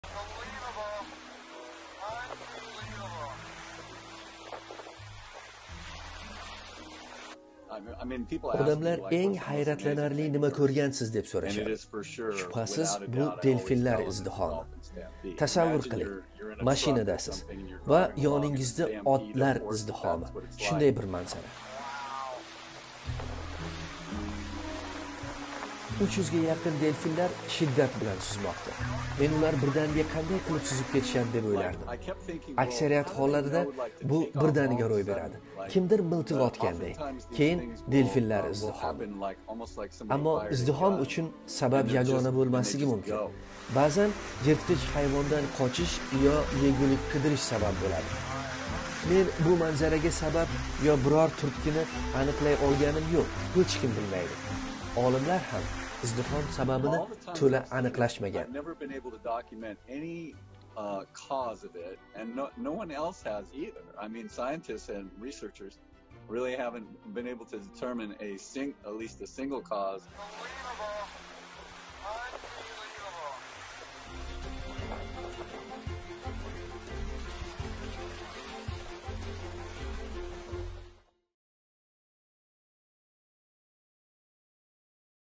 Дунё, табиат: Делфинлар издиҳомини кўрганмисиз? - BBC News O'zbek
Дунё, табиат: Делфинлар издиҳомини кўрганмисиз? -  BBC News O'zbekBBC News O'zbek янгиликлар каналига хуш келибсиз!